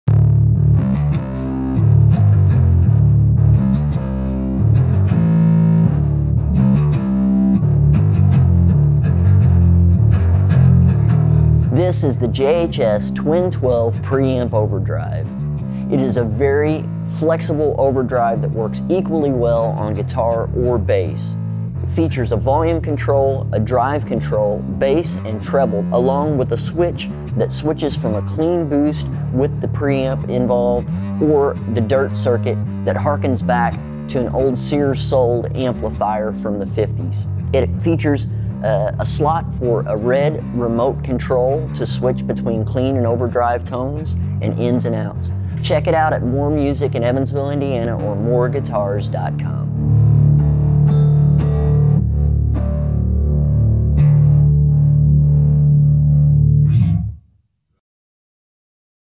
JHS Twin Twelve V2
The Twin Twelve channel drive is the first ever all-analog stompbox to replicate the classic tones of the Silvertone 1484 "Twin Twelve" amplifier. The pedal’s sound and response are meticulously hand-tuned to achieve a striking similarity to the tones made famous by players such as Jack White, Beck, Coldplay, and others. But that’s just the beginning. With its ability to go from a spanky clean preamp boost to a classic growling medium/heavy overdrive, the “Twin Twelve” V2 is a great option for the do-it-all overdrive spot on your pedalboard.